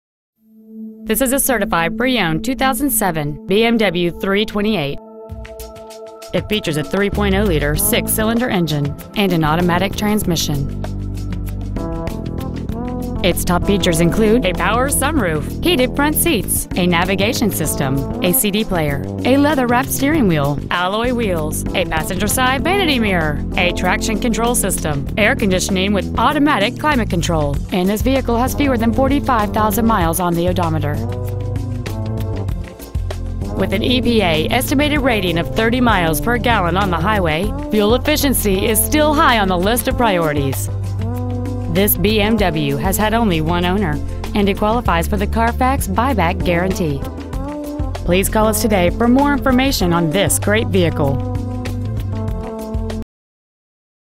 Pre-Owned 2007 BMW 328 Monrovia CA
Year: 2007
 Make: BMW
 Model: 328
 Engine: 3.0L I-6cyl
 Trans.: Automatic
 Exterior: Gray
 Miles: 44,963
 Interior: Black

 1425 S. Mountain Ave.

 2007 BMW 328 Monrovia CA